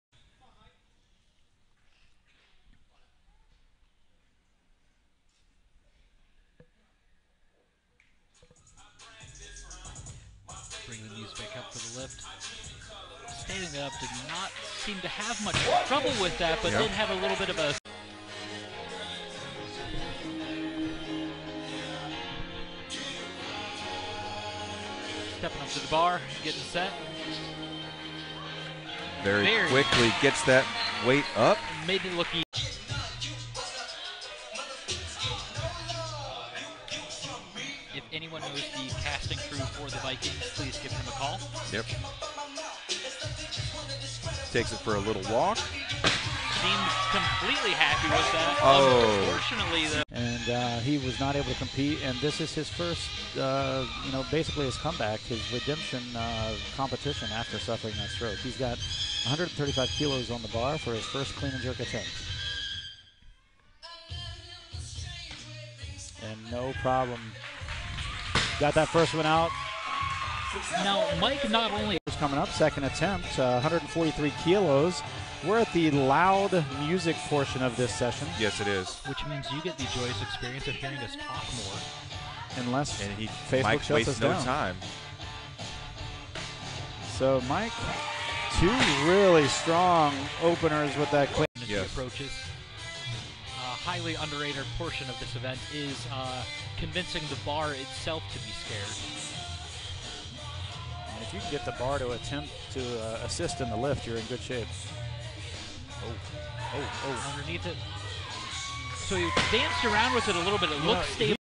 Mike Walters - 2018 Strongest Unicorn
Mike Walters, owner of Chalkdust Crossfit (Powell, Ohio), competing in the 2018 Validus Strongest Unicorn olympic weightlifting competetion hosted by Project Lift and Endeavor Defense & Fitness. Mike's snatches were 115kg (pass), 120 kg (pass), 125 kg (judges deemed unsucessful); and cleans were 135kg (pass), 143 kg (pass), 150 kg (pass).